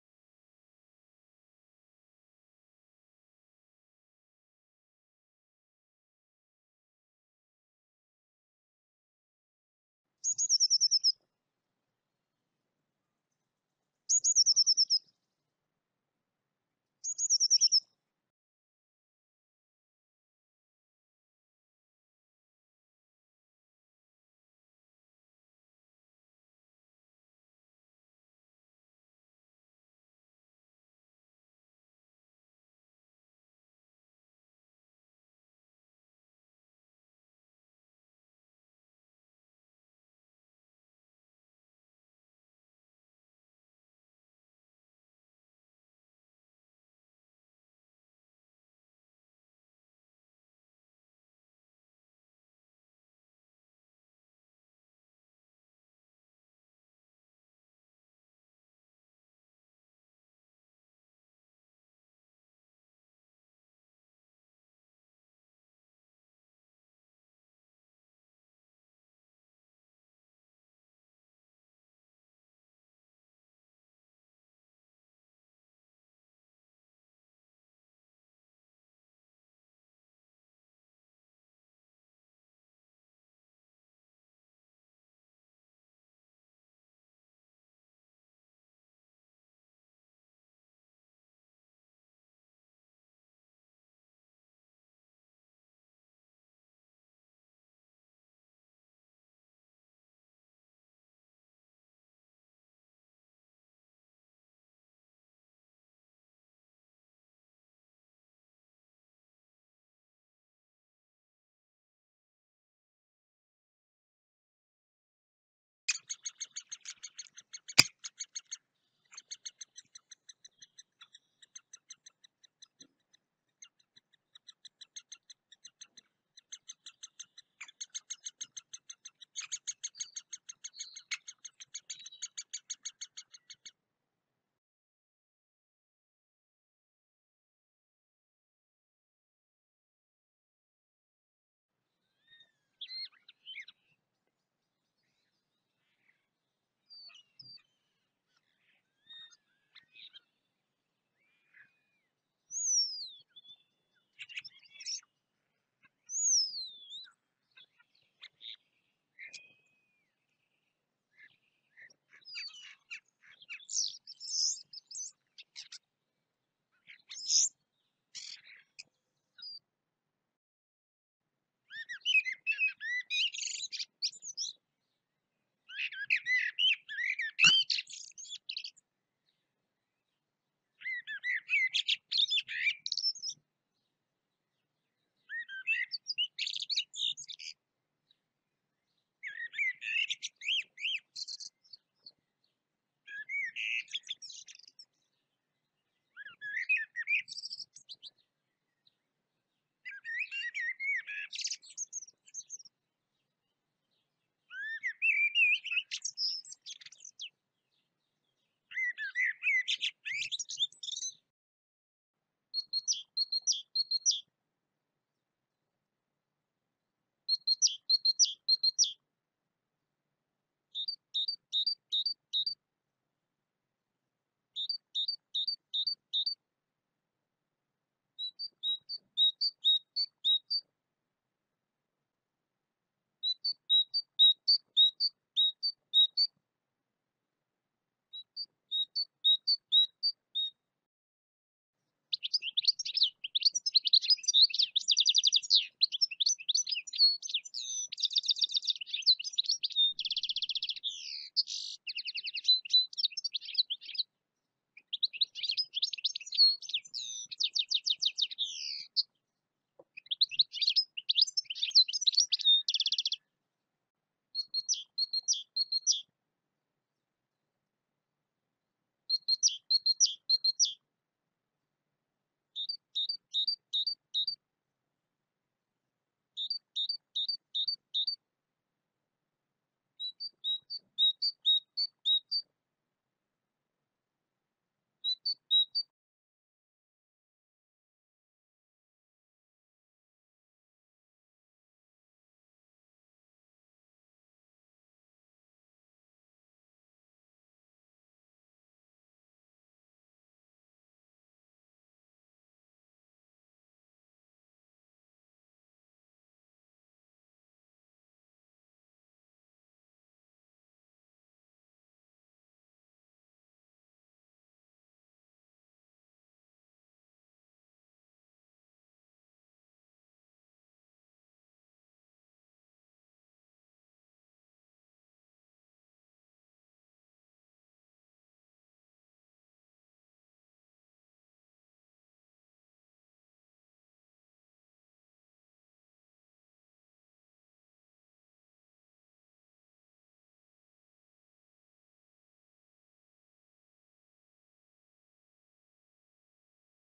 Adaptări ale viețuitoarelor la condiții de viață din diferite medii - Investigație
Materialul realizat  prezintă informații despre câteva specii de păsări observate în orașe dinTransilvania.
 -  ,,Păsările se adaptează la mediul de viață din aglomerările urbane?” a fost ipoteza de la care am pornit realizarea resursei.

Observațiile realizate au avut în vedere obținerea informațiilor despre penajul, aria de răspândire, încadrarea în categoria specie sedentară/ migratoare, zona pentru cuibărit sau hrană preferată de păsările prezentate.

Am prezentat detalii despre ciocănitoare, graur, pițigoi mare, mierlă și sticlete..Diversitatea speciilor de păsări este ilustrată printr-o serie de imagini sugestive și înregistrări audio ale trilurilor acestora. Pe baza observațiilor înregistrate am concluzionat că speciile de păsări s-au adaptat mediului de viață din orașe.

Autor: Codrut Gabriel Morariu